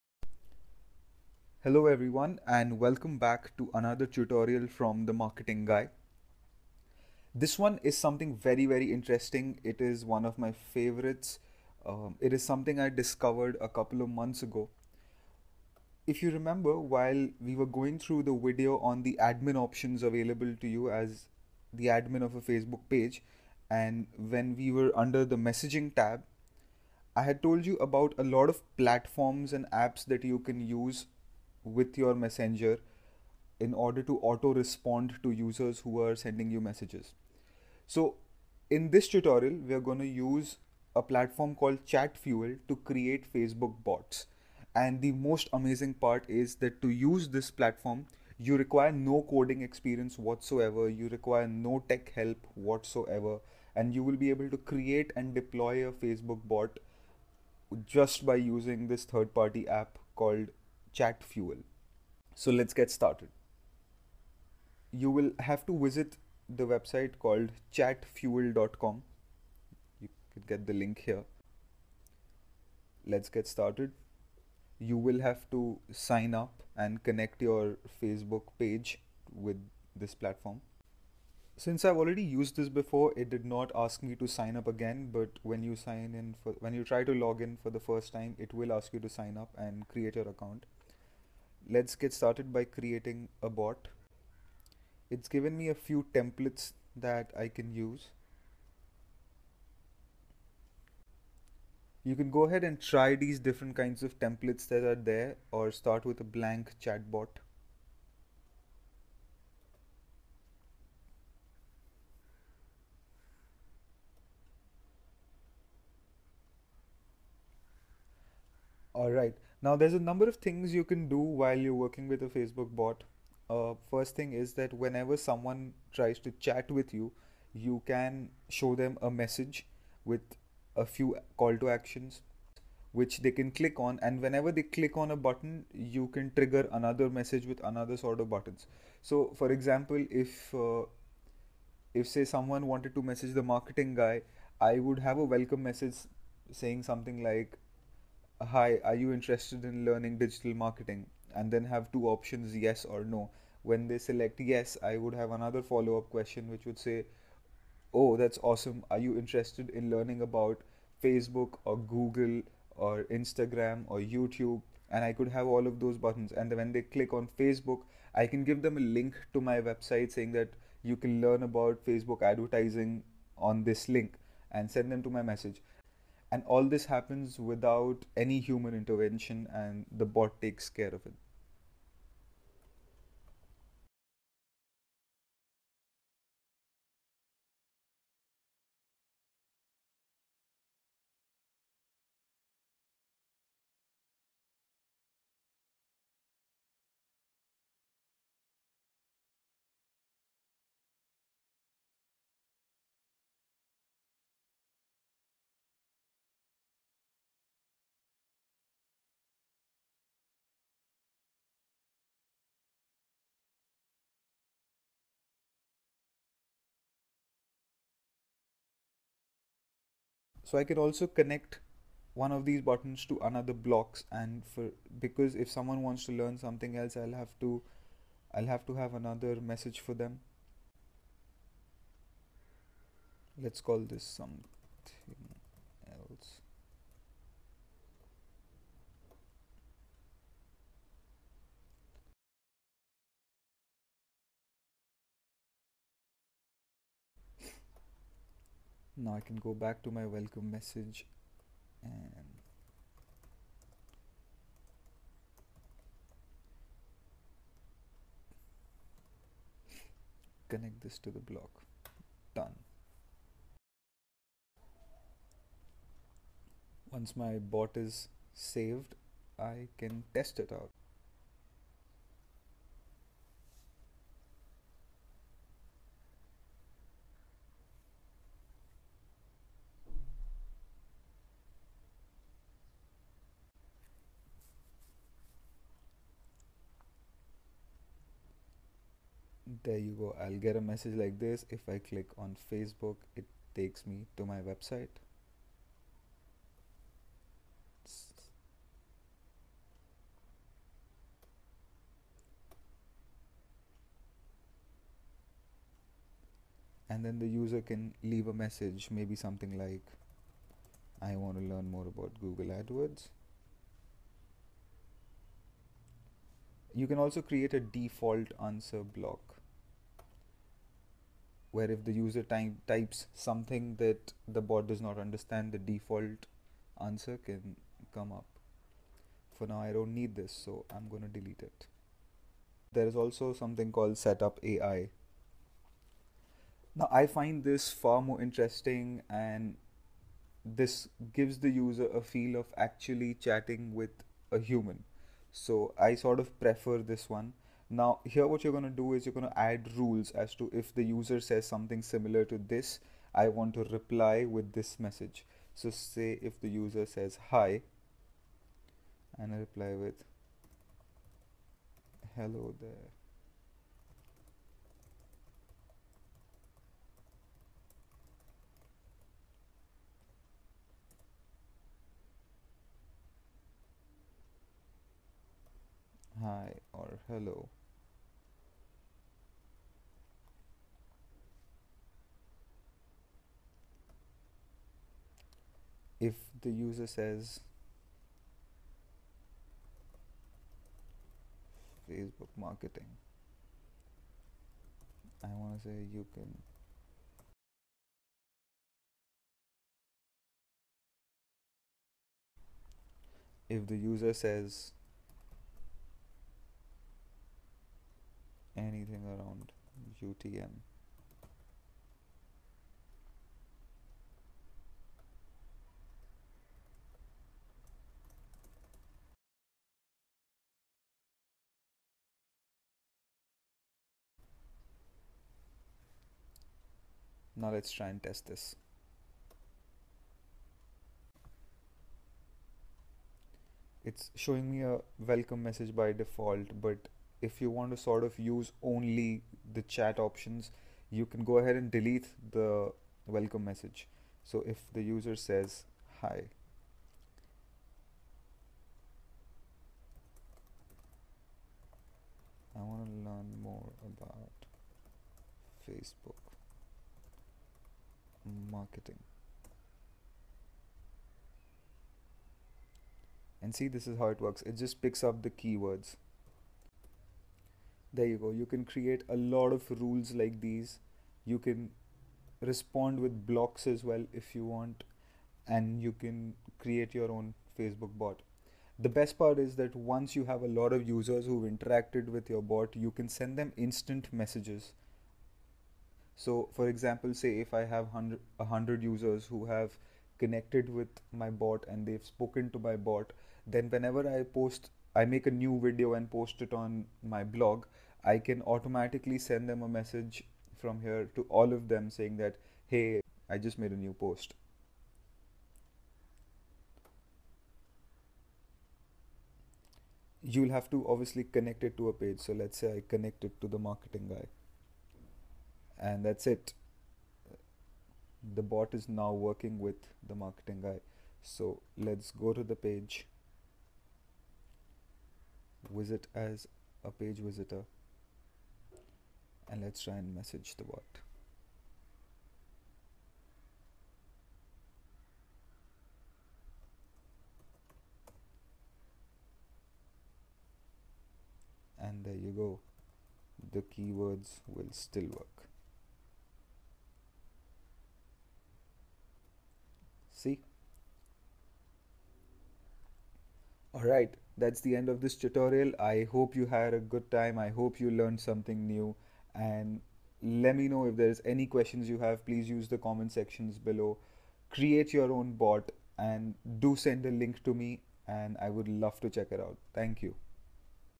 Creating A Facebook Messenger Bot Without Coding
In this tutorial, you will learn to create facebook messenger bots without any coding. We will be using a third party for this called Chatfuel.